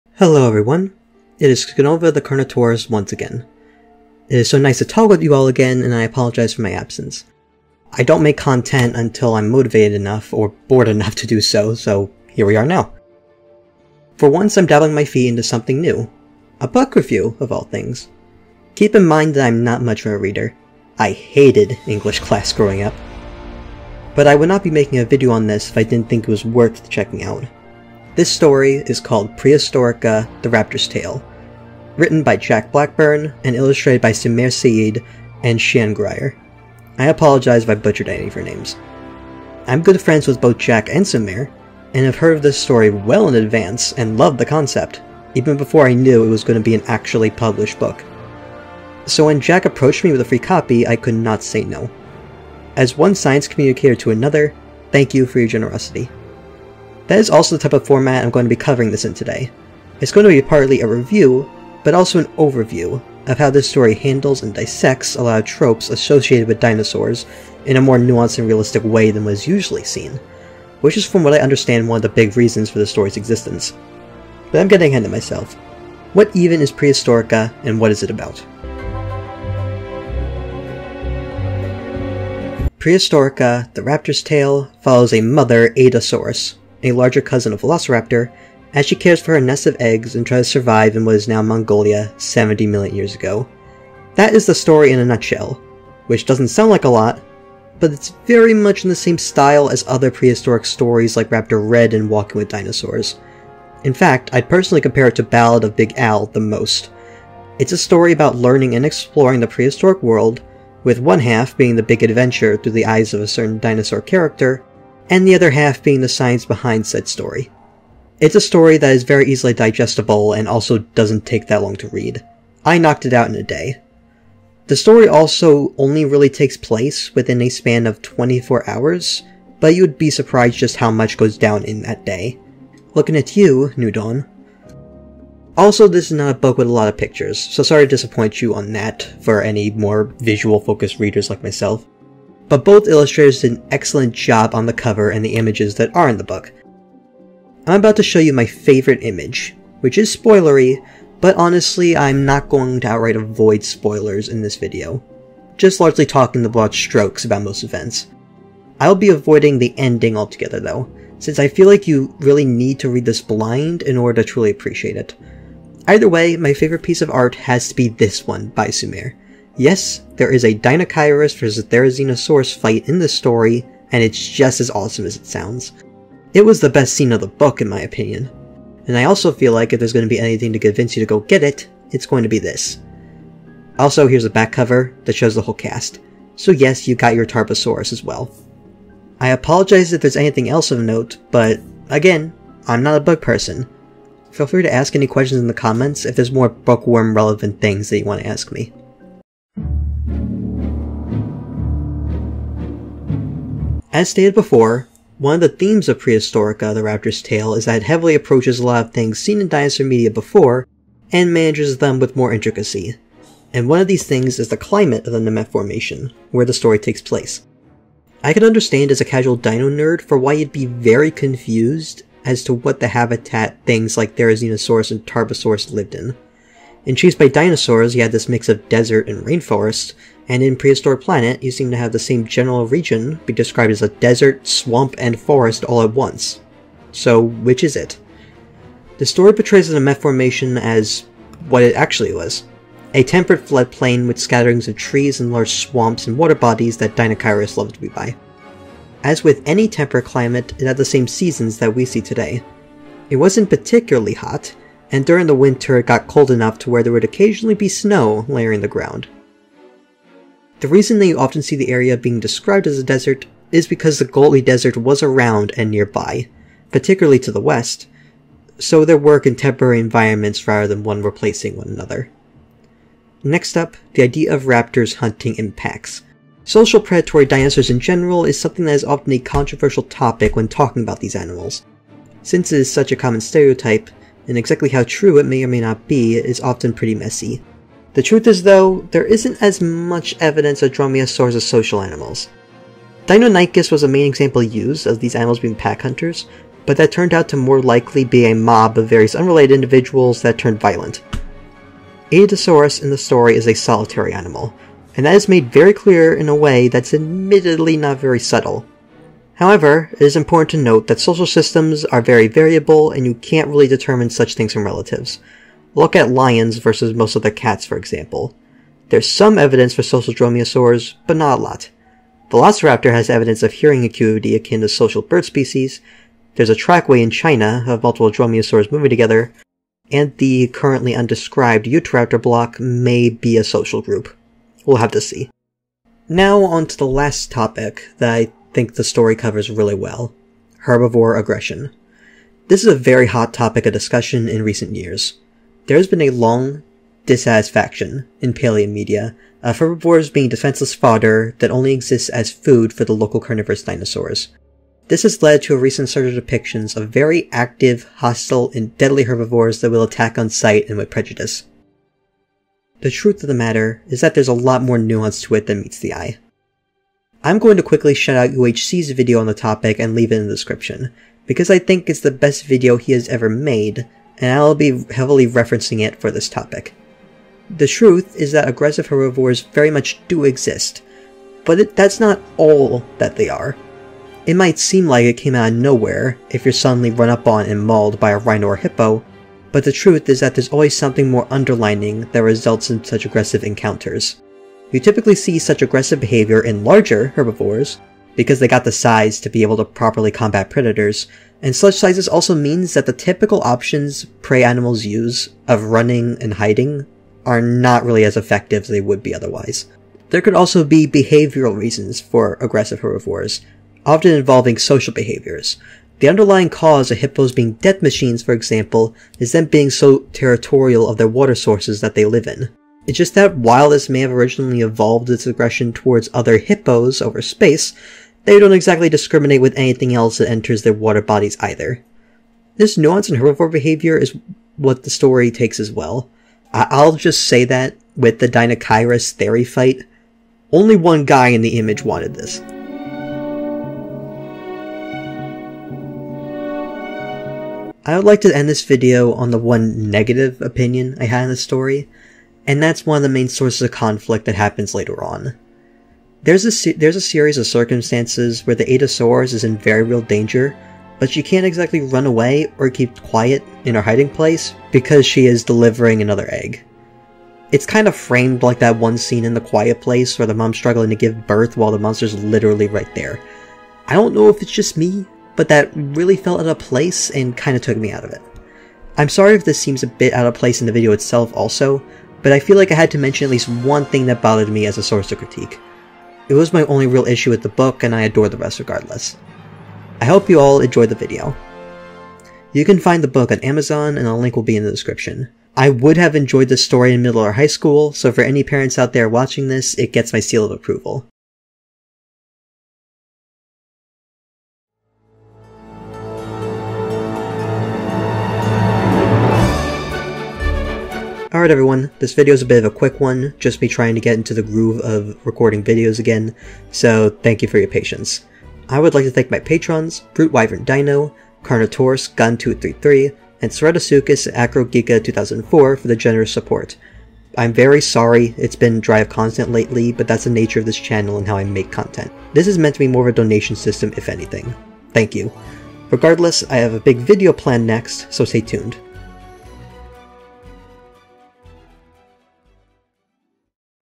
Prehistorica: The Raptor's Tail - A Book Review and Analysis
Big thanks to my good friend Blackburn for the free copy. This video was a joy to make. I actually had this video ready for about a month now, planning to release it as a two-parter with another video. However, youtube is insisting that I have an adblocker (when I don't) and as such currently has me in a stranglehold. So I'm not really going to be making any more vids as my own kind of protest until they leave me tf alone. That's what the end-credits bit is about. I'll see yall later. I got big plans whenever youtube get its shit together.